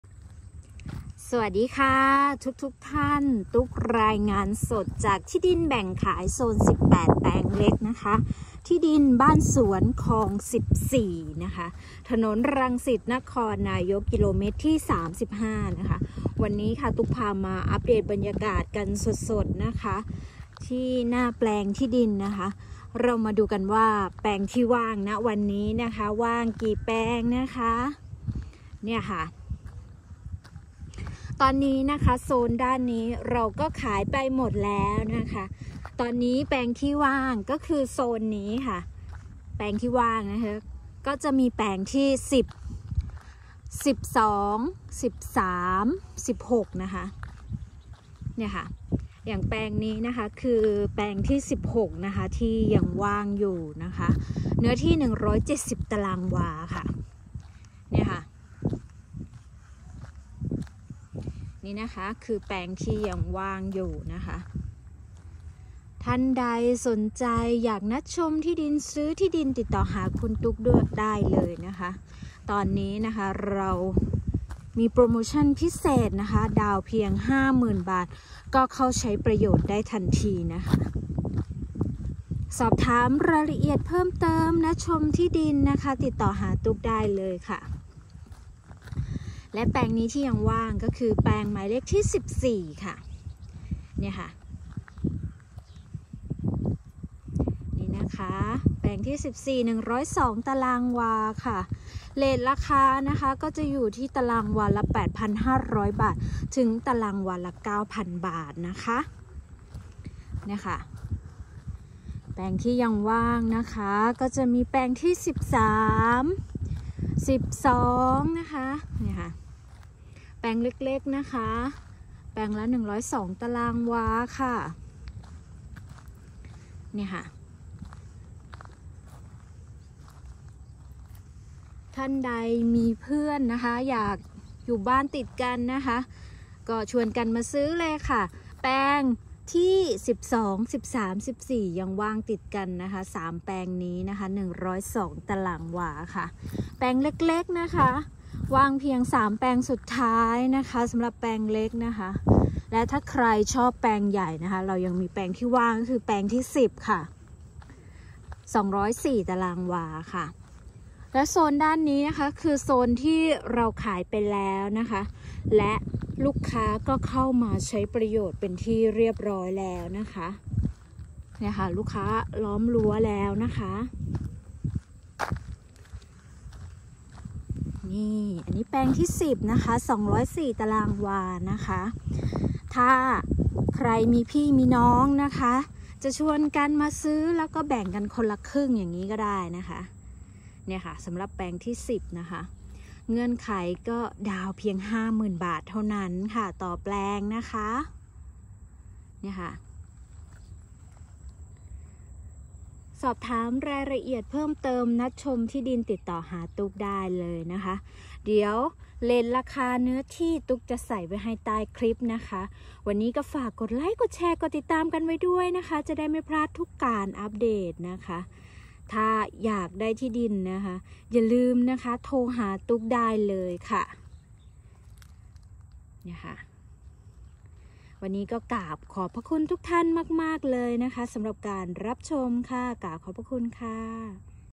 ที่ดิน คลอง 14 อากาศเริ่มเย็นแล้ว เราไปชมแปลงว่างกันได้เลยค่ะ l 0992216289 คุณตุ๊ก
Update 14 พฤศจิกายน 2564

ท่านใด สนใจที่ดิน ยังมีแปลงว่างอยู่นะคะ

ยินดีต้อนรับสู่ จังหวัดนครนายก

สวัสดี เดือน พฤศจิกายน 2564

โครงการ 🏡
ที่ดิน บ้านสวนคลอง14
เฟส 1 แปลงเล็ก
อัพเดทแปลงว่าง ‼️

📌ต.คลองใหญ่
อ.องครักษ์ จ.นครนายก

ถนนรังสิต-นครนายก กม.35
เข้าซอยข้างร้าน Bitter*Sweet
 ประมาณ 2 กิโลเมตร

1️⃣0️⃣แปลง 10 เนื้อที่ 204 ตารางวา
= 1,836,000 บาท

1️⃣2️⃣แปลง 12 เนื้อที่ 102 ตารางวา
= 867,000 บาท

1️⃣3️⃣แปลง 13 เนื้อที่ 102 ตารางวา
ตารางวาละ 8,500 บาท = 867,000 บาท

1️⃣4️⃣แปลง 14 เนื้อที่ 102 ตารางวา

1️⃣6️⃣แปลง 16 เนื้อที่ 170 ตารางวา
= 1,513,000 บาท

🅰️ A1 เนื้อที่ 488 ตารางวา
= 3,416,000 บาท

🅱️ B5 เนื้อที่ 485.3 ตารางวา
= 3,397,100 บาท

🅱️ B7 เนื้อที่ 485.3 ตารางวา
= 3,397,100 บาท

⭐️ดาวน์น้อยเริ่มต้นเพียง 50,000 บาท ขึ้นไป
หรือ 30% ของราคาที่ดิน

หรือซื้อสด จอง 100,000 บาท
โฉนดพร้อมโอน